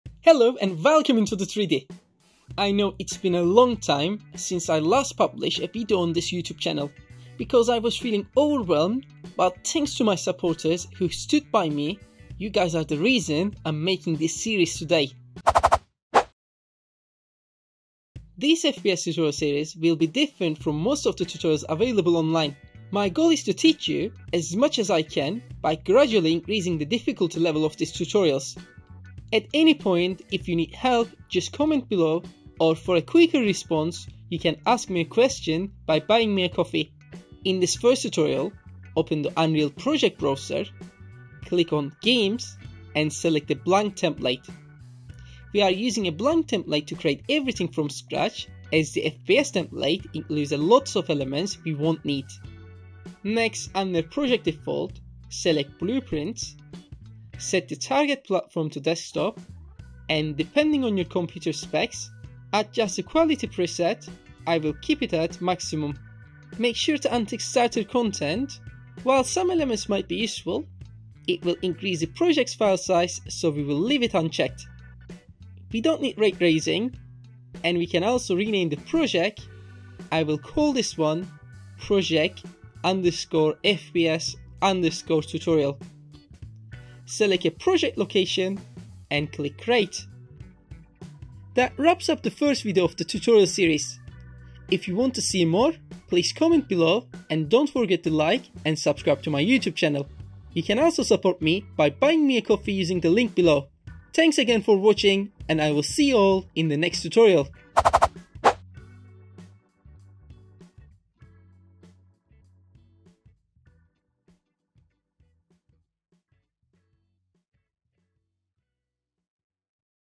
Unreal Engine FPS Tutorial for Beginners | 1- Project Setup
Welcome to the first video of Unreal Engine FPS Tutorial for Beginners.